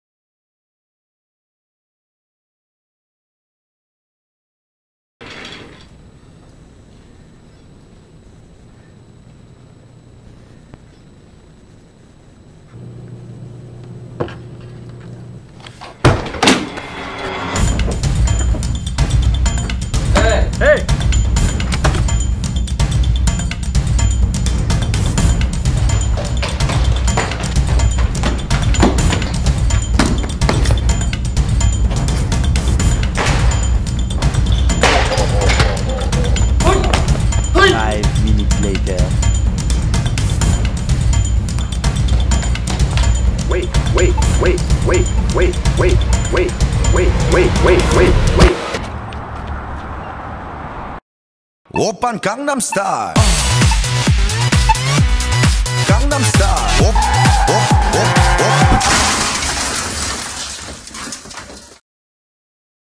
Escape from: Dention VS Tution (Full Scholarship)
This is a comparison showing the similarities between Escape from Dention and Escape from Tution which were made 8 years apart from each other. In making of Tution, efforts were made to preserve the timing of some the sequences found in Dention. Several parts were trimmed out of Tution for the sake of comparision.  It is recommended to watch the original movies first.